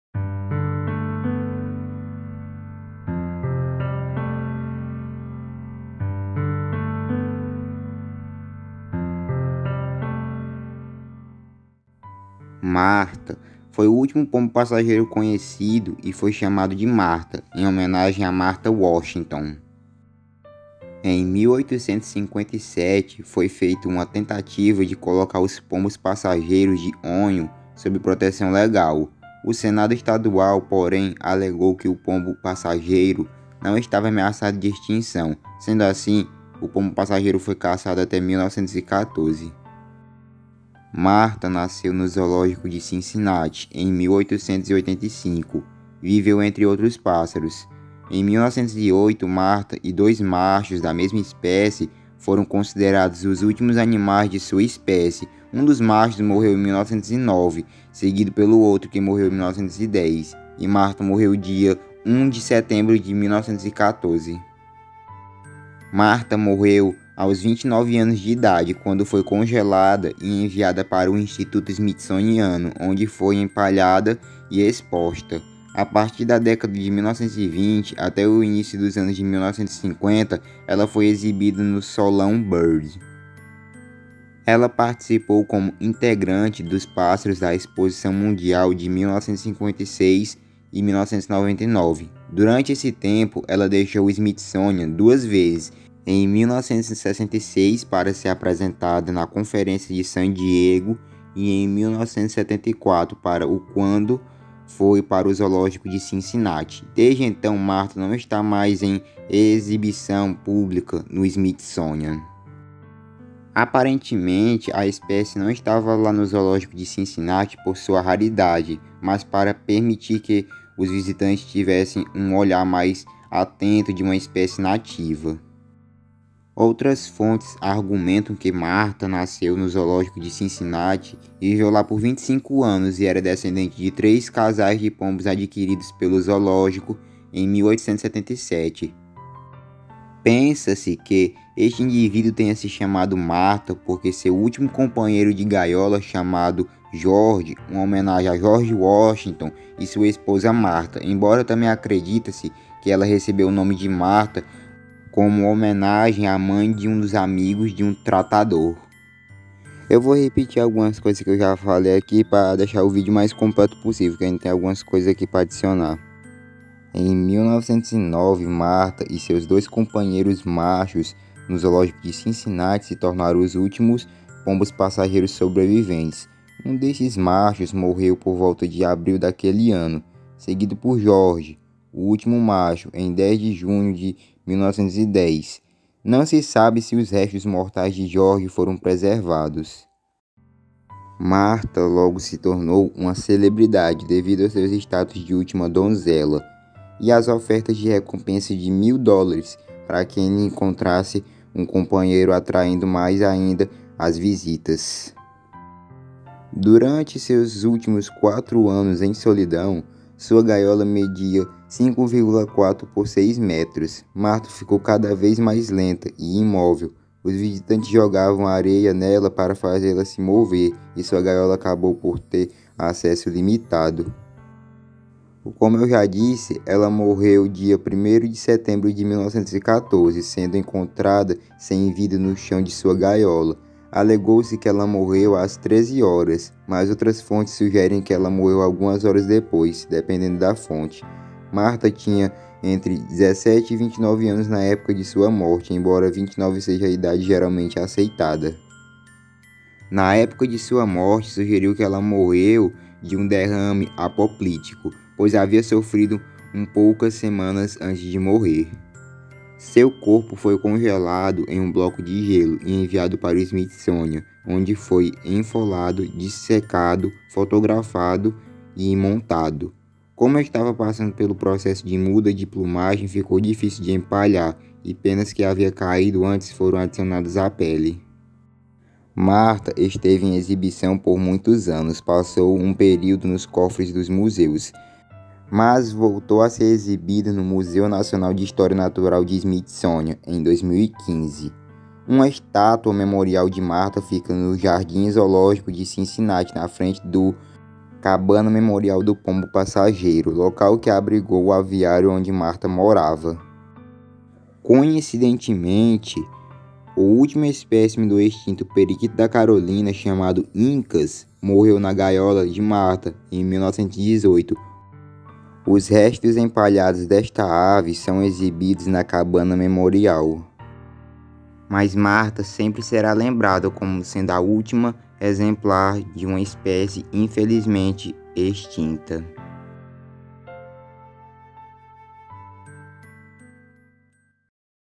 Martha o Último pombo passageiro (Vídeo Homenagem).
Informações

Espécie

Pombo-passageiro

Sexo

fêmea

Nascimento

1885

Local de nascimento

Cincinnati

Morte

1 de setembro de 1914

Local de morte

Zoológico de Cincinnati

Martha (Zoológico de Cincinnati, 1885 — 1 de setembro de 1914) foi o último pombo-passageiro conhecido, e foi chamada de "Martha" em homenagem a Martha Washington.